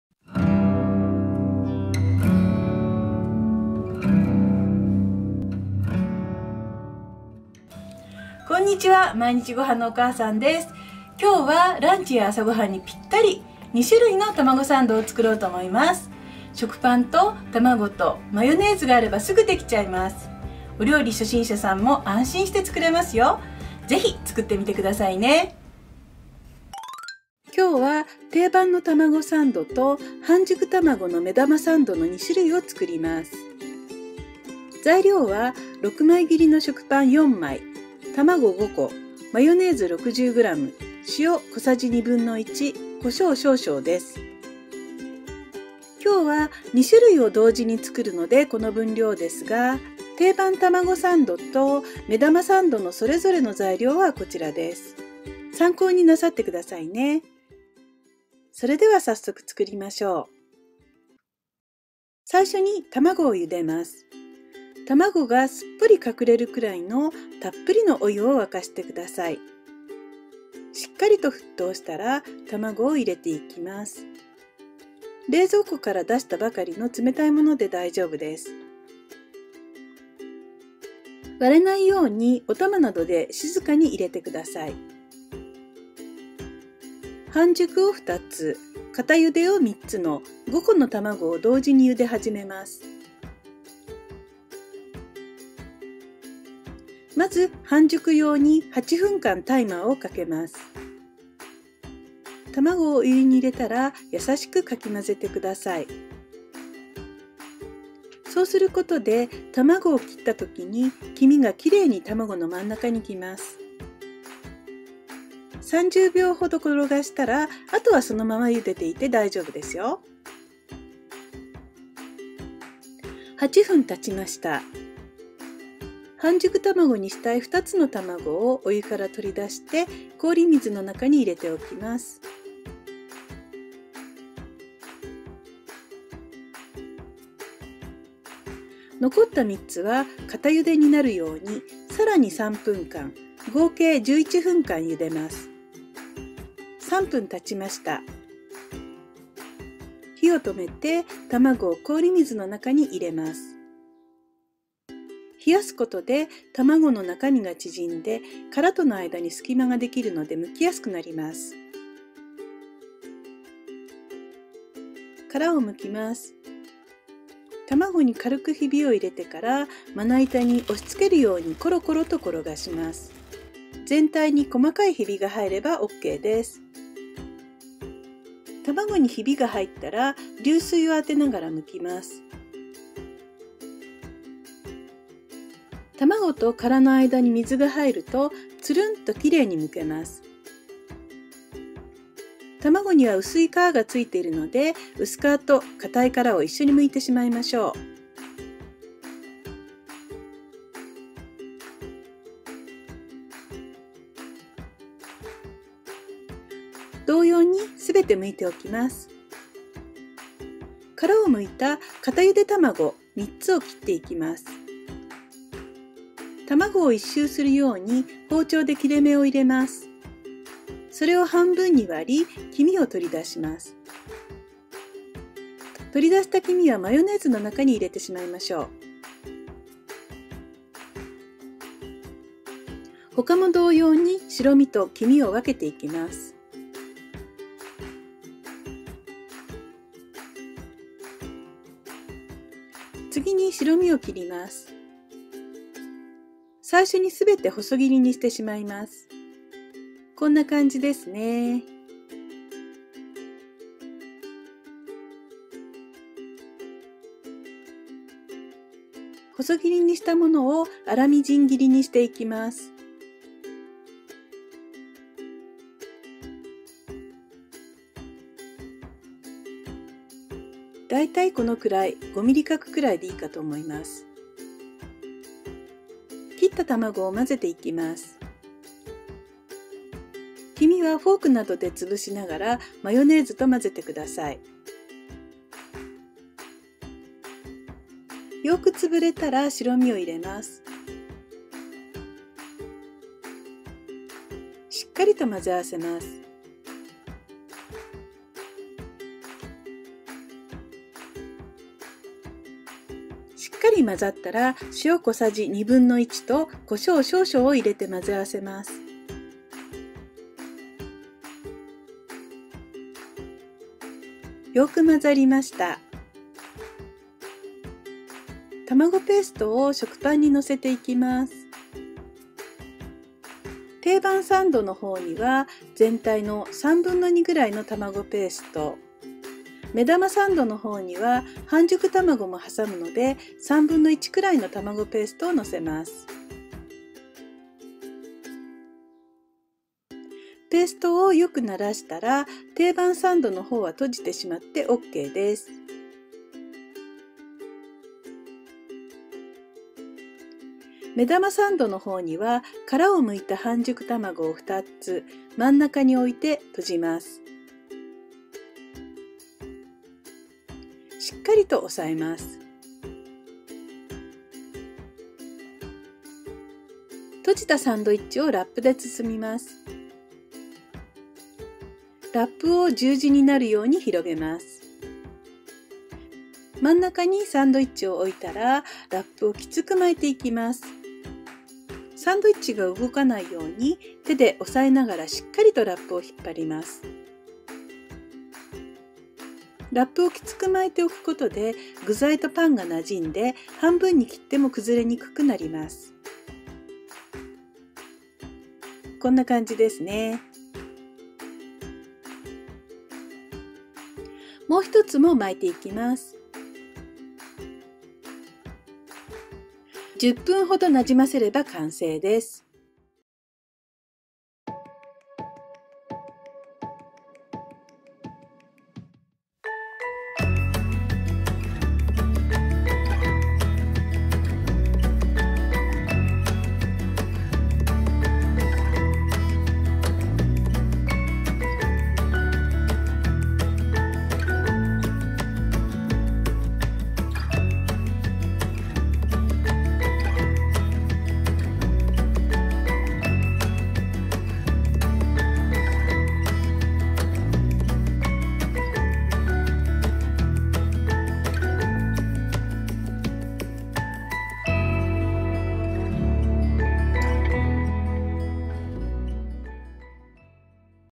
【ゆで卵の作り方完全マスター】ふわふわ美味しい２種類のたまごサンドを作りながらゆで卵の作り方をマスターしよう！
固ゆで卵と半熟卵の茹で方から剥き方まで、コツをご紹介します。覚えておくと便利ですし難しいことはないのでぜひこの機会にチャレンジしてみてくださいね。
サンドイッチもシンプルなのに卵のうまみたっぷりでどんどん食べれちゃいます。大人の方は隠し味に粒マスタードなどを入れても美味しいかもしれません。

２種類のたまごサンド
・食パン（６枚切り）　４枚
・卵　５個（固ゆで３、半熟２）
・マヨネーズ　６０g

定番たまごサンド
・食パン（６枚切り）　２枚
・卵　２個（固ゆで）
・マヨネーズ　４０g

半熟卵の目玉サンド
・卵　３個（固ゆで１、半熟２）
・マヨネーズ　２０g